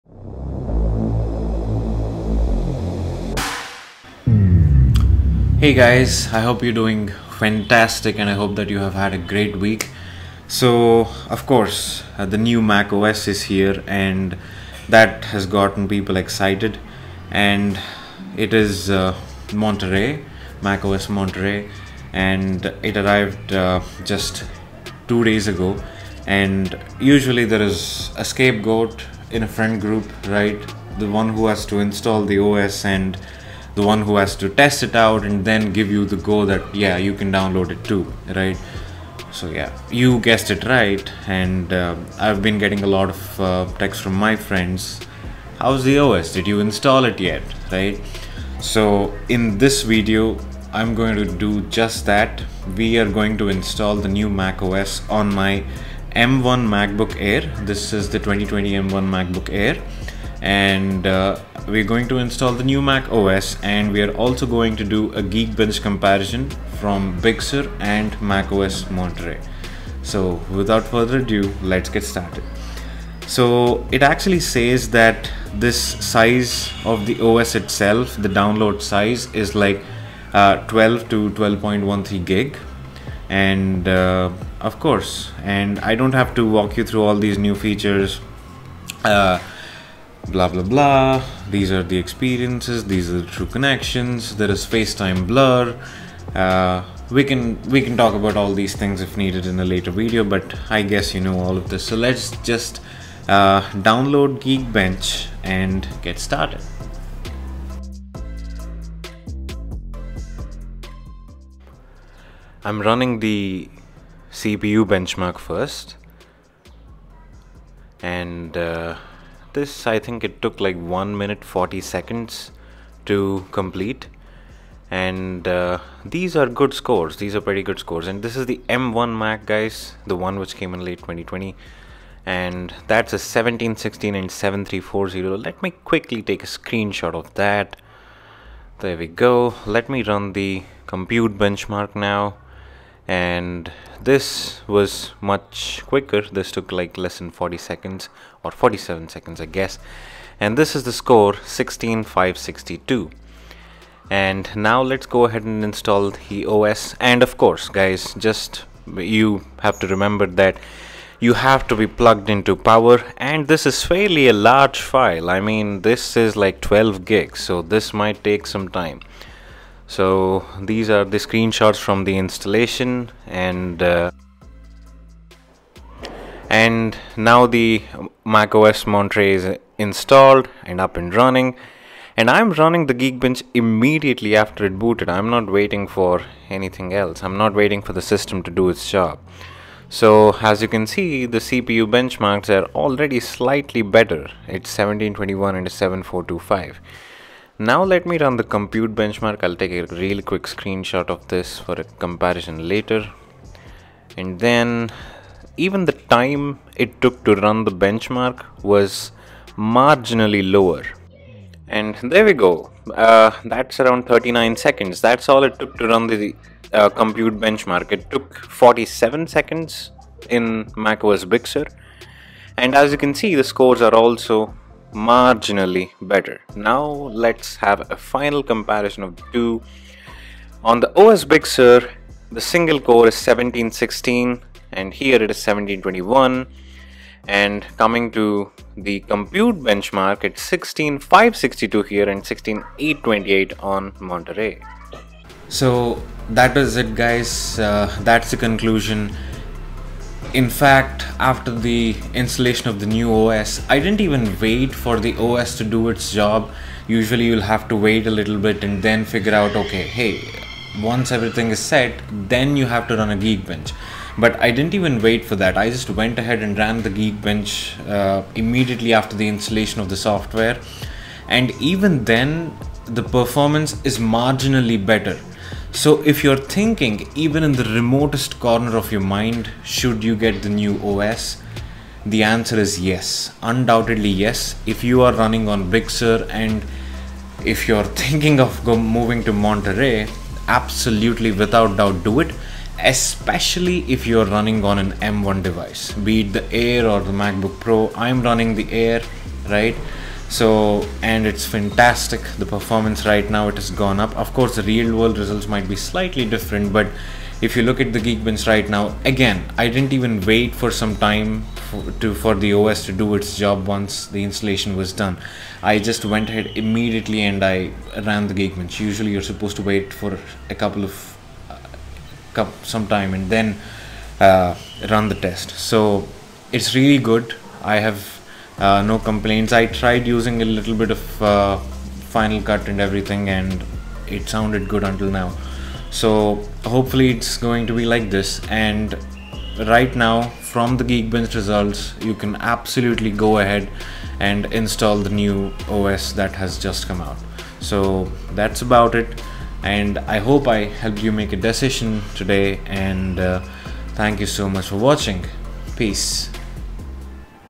Mac OS Big Sur Vs Mac OS Monterey | Geekbench Test on Mac Book Air M1 chip
In this video, we are doing a Geekbench test on the M1 MacBook Air that came out in late 2020. There's going to be test of the CPU and the Computation power. And I have give you a spoiler alert - You won't be disappointed.
This is going to be a test of the performance, speed and various other parameters that matter to most consumers. You know what a Geekbench test is - I probably won't have to tell you :)

I hope I help you make a decision today. And have a lovely day ahead ! Peace !